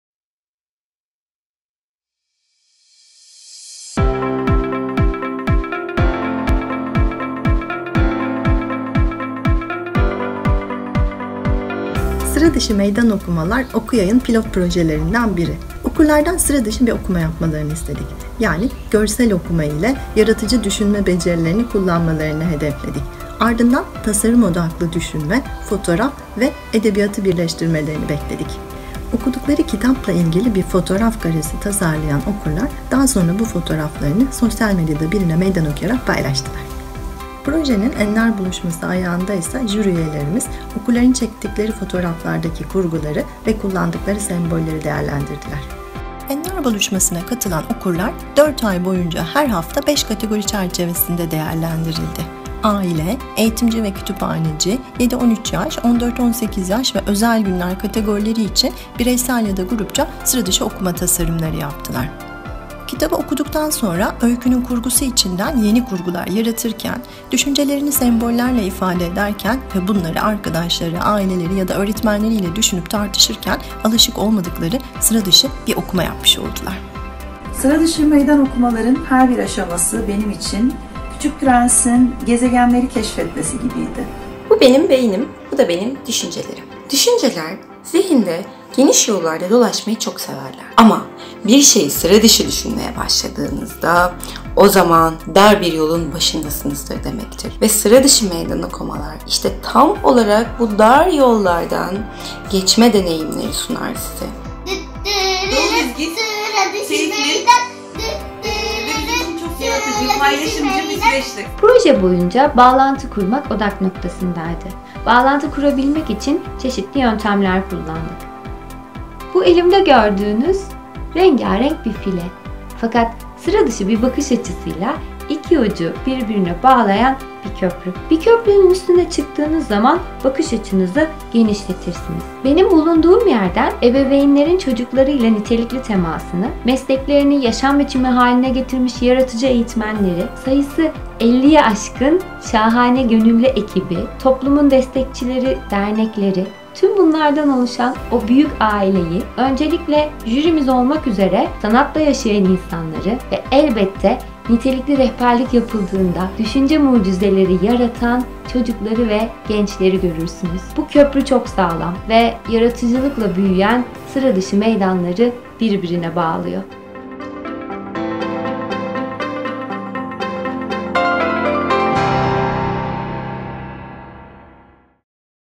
SIRADIŞI MEYDAN OKUMALAR PROJESİ TANITIMI
Pilot proje olarak İstanbul’dan hibe almaya hak kazanan projelerden biri de Sıra Dışı Meydan Okumalar projesi oldu. Proje “kitap her yerde ve her koşulda okunur” ve “okumak eğlencelidir” düşüncelerinden yola çıkan bir meydan okuma yarışması. Yarışmaya katılmak için sıra dışı yerlerde gerçekleştirilen okuma eyleminin okunulan kitabın adına ve içeriğine uygun şekilde bir fotoğraf karesinde görselleştirilmesi gerekiyor. Bu görseller yarışma için belirlenen etiketlerle sosyal medyada yarışmaya katılan okurlar tarafından paylaşılıyor. Önceden oluşturulmuş bir halk jürisi bu görselleri değerlendirdikten sonra kazanan yarışmacılar çeşitli ödüller kazanıyor.

Projenin yaş grubu geniş bir yelpazeyi kapsıyor. Yarışmada 0-6 yaş; 7-13 yaş; 14-18 yaş; 18+ yaş; özel gün; öğretmen ve aile olmak üzere farklı kategoriler bulunuyor. Özel gün, öğretmen ve aile kategorileri yaş gruplarından bağımsız olarak değerlendiriliyor. Bireysel ve grup katılımına da olanak veren bu iki kategori okuma kültüründe önemli yeri olan aileyi ve öğretmeni sürece farklı bir kategoride dahil ederek okuma sürecine katılımlarını teşvik ediyor. Özel günler kategorisi için ise sıra dışı okuma fotoğraflarının yarışmadan sonra da kullanılması, kitapla doğrudan ilgili olan ya da olmayan özel günlerde de konuyla ilgili farkındalık oluşturulması amaçlanıyor.

Projeye başarılar, tüm katılımcılara iyi eğlenceler ve güzel okumalar diliyoruz.